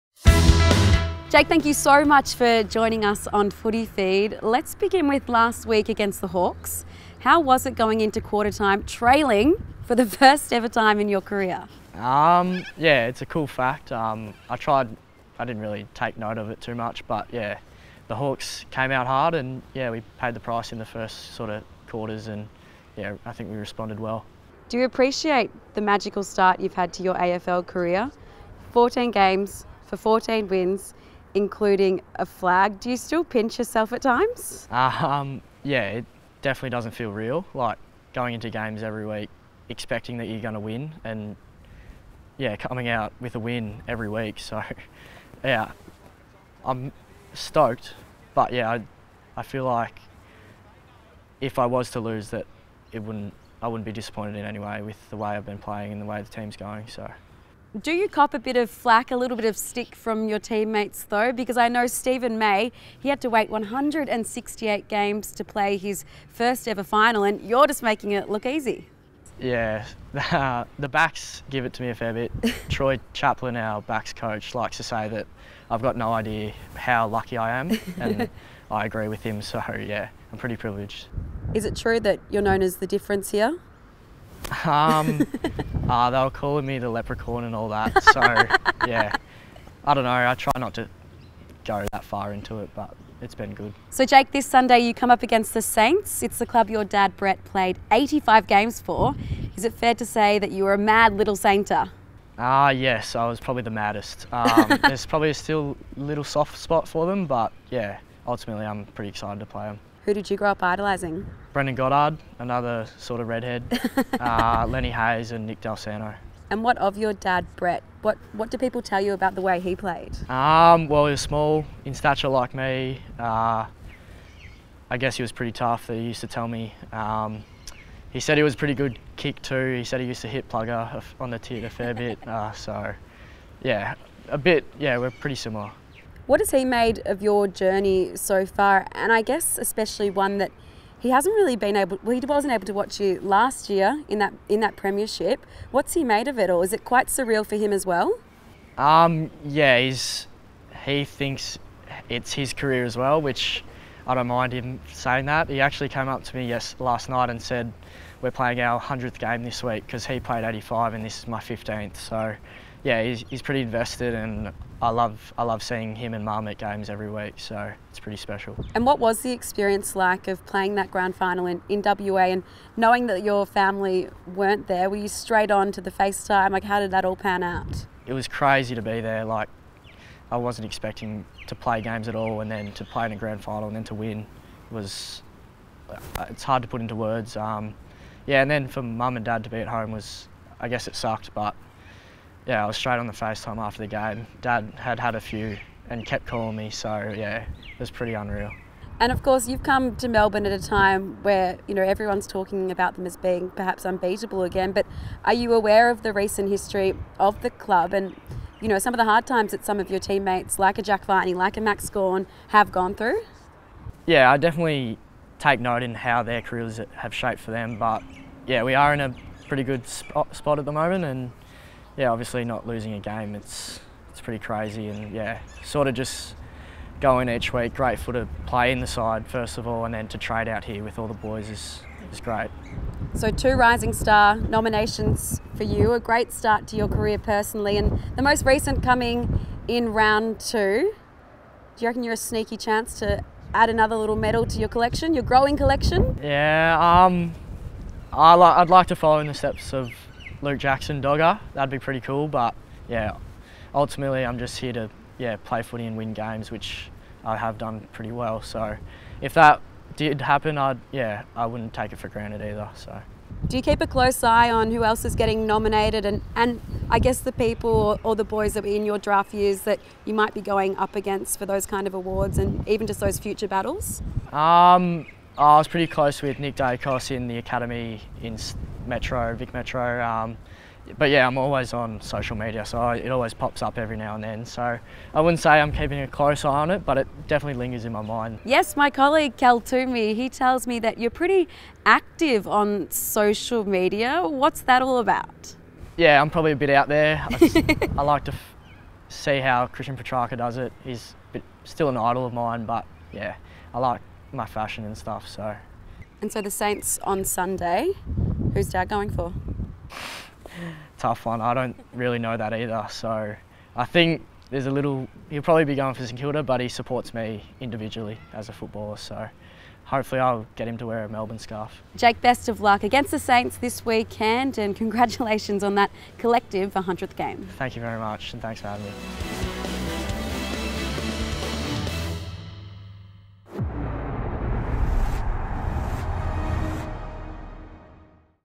AFL Footy Feed | Jake Bowey
Young Demon Jake Bowey chats to AFL Footy Feed on his career to date